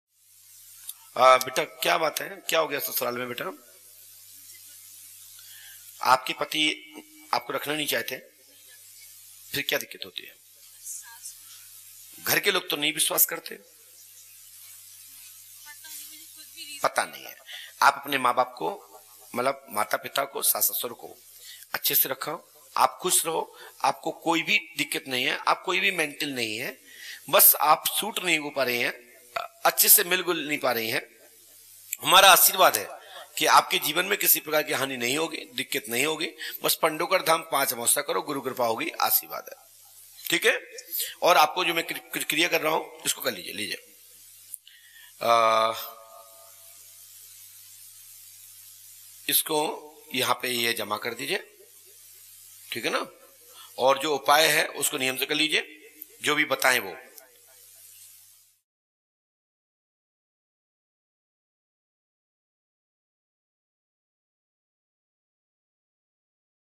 दिव्या दरबार आए पिता अपनी बेटी की समस्या को लेकर ।। Pandokhar Dham Sarkar ।। Aastha Bhajan
दिव्या दरबार आए पिता अपनी बेटी की समस्या को लेकर